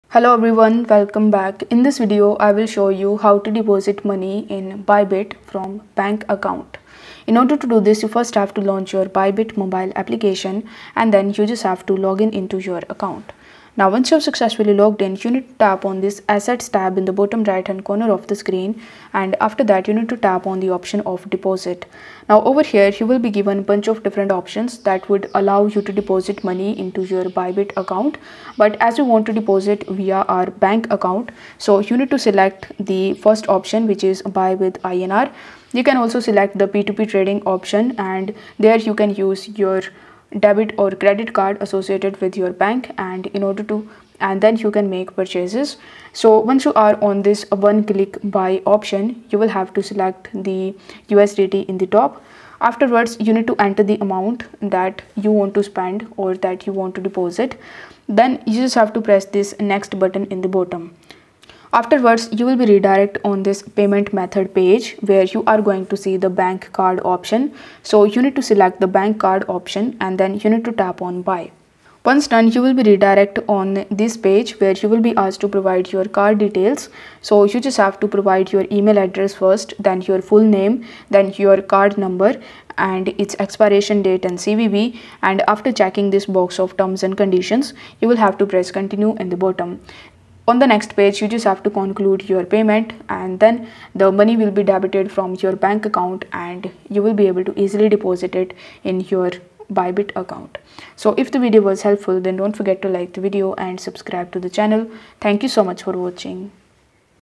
How to Deposit Money in Bybit From Bank Account 2024
Welcome to Token Tutorials! In each video, I will show you How to Deposit Money in Bybit From Bank Account 2024